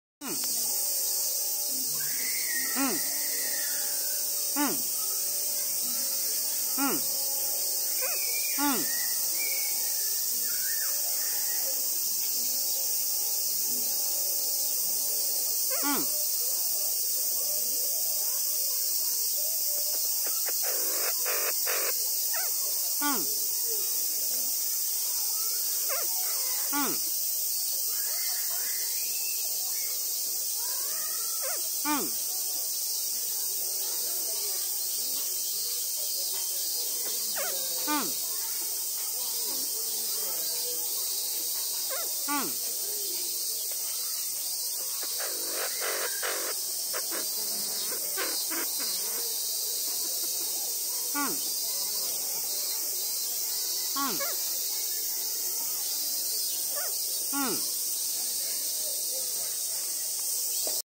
"コシジロヤマドリとの会話"
日本に古来からいるコシジロヤマドリ。
こちらのヤマドリさんは埼玉県の大宮公園小動物園におります。
いつも「うん、うん。」と鳴いています。
こちらもうん、ふんと言うと呼応してくれます。
興奮すると「ギャーギャー」と鳴きます。